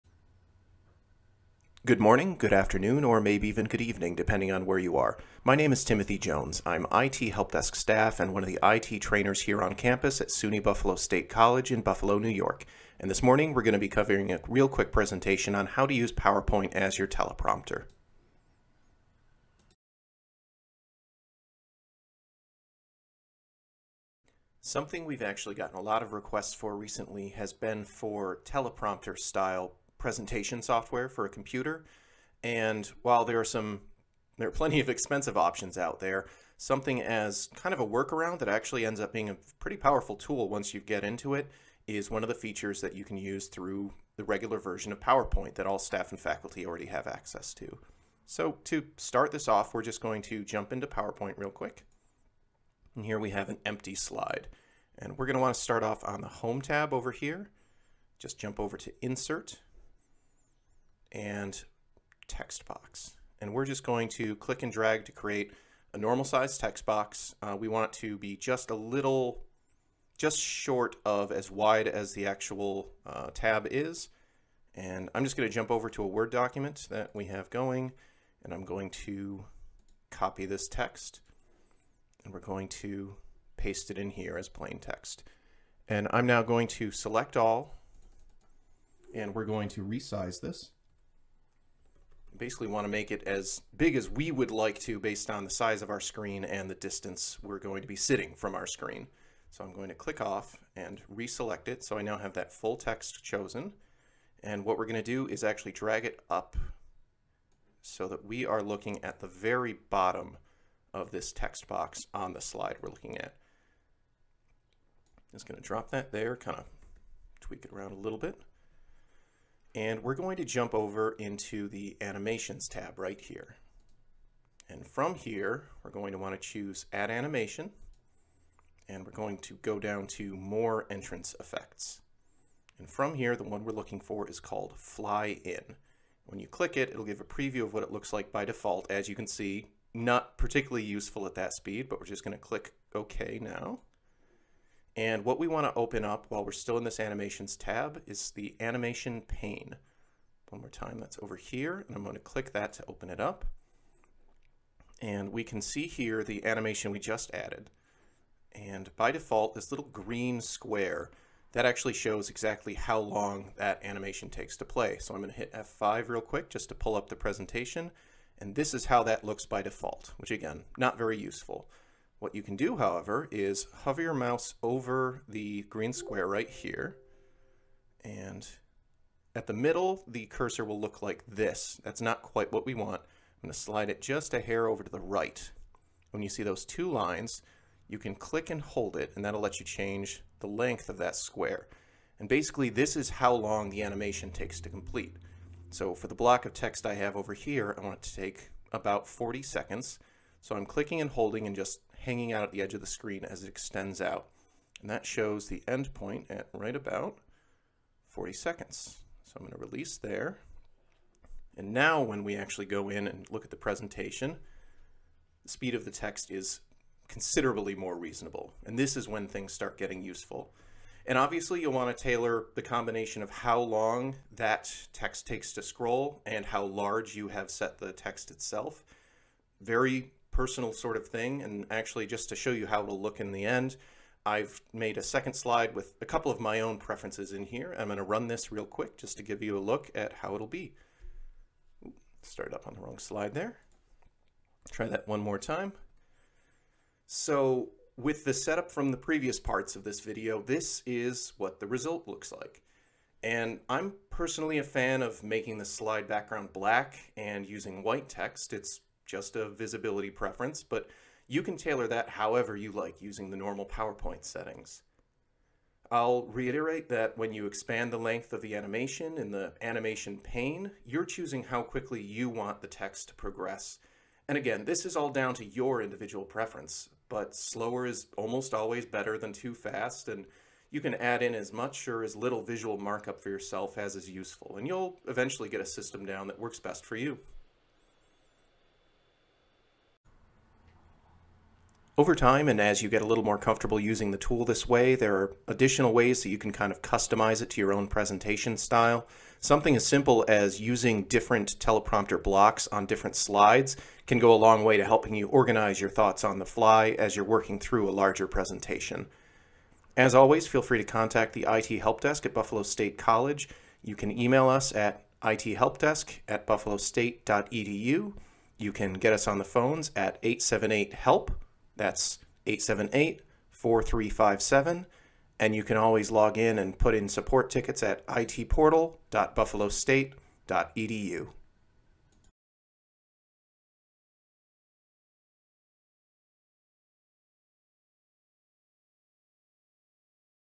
PowerPoint as Your Teleprompter - Expanding your digital toolbox
Please note that the resulting scroll motion is smooth in person. The stutter/stagger visible in the video is the result of video compression/conversion.

Written directions included here in Steps 1-6:

1. On a blank slide, create a Text Box about the size of the slide’s visible area (leaving some space on the left/right-hand sides)

2. Type or paste in the block of text that you’d like for the text-scroll, and “Select All” to resize it as large as you’d like it to appear.

3. When increasing the font size in step 2, the height of your Text Box will start extending below the visible slide area. You want to reposition it so the *bottom* (end) of that Text Box rests inside the slide.

4. Be sure your Text Box is selected, then add the “Fly In” animation to it.

5. Open the Animation Pane, and hover your mouse over the right-hand edge of the green animation timing square for “Fly In” on the Pane’s timeline. Your cursor should turn into an L/R arrow with two vertical lines, to represent that you can click-and-hold, and extend the duration of the animation. You want to stretch that green square to the right, setting the duration you would like for your full reading.

6. You can now present that slide, and get a feel for the size/speed of the resulting text scroll. Any of the previous settings can be adjusted to suit your taste and presentation.